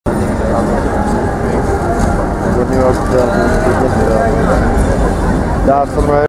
Vivienne van den Assem zwaait naar msbronwater !!!
msbronwater bij turn on the lights op de dam